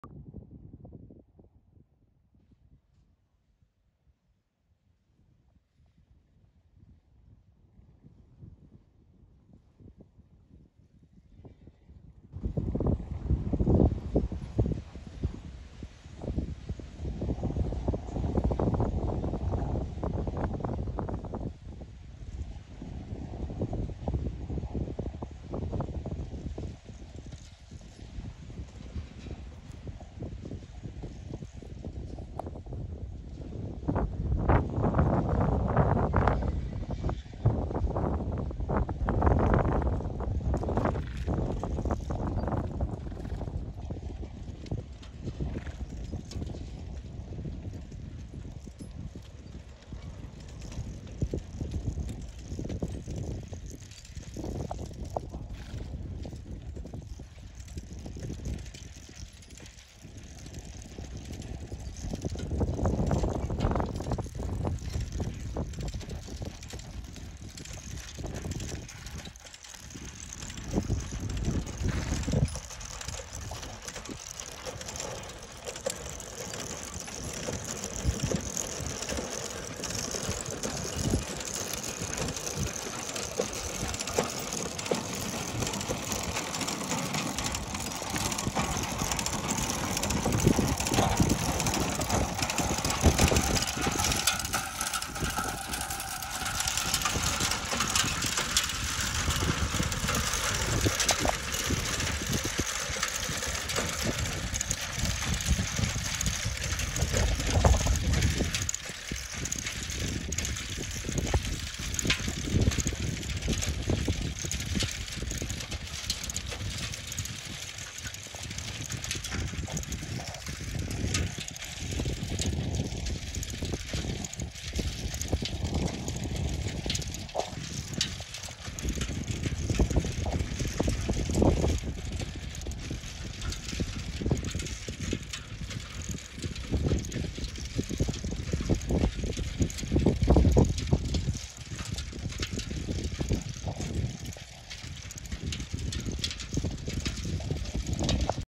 Heavy Horse#Manure#dung spreading#soil fertility#Percheron
Spreading very dry dung on the pasture.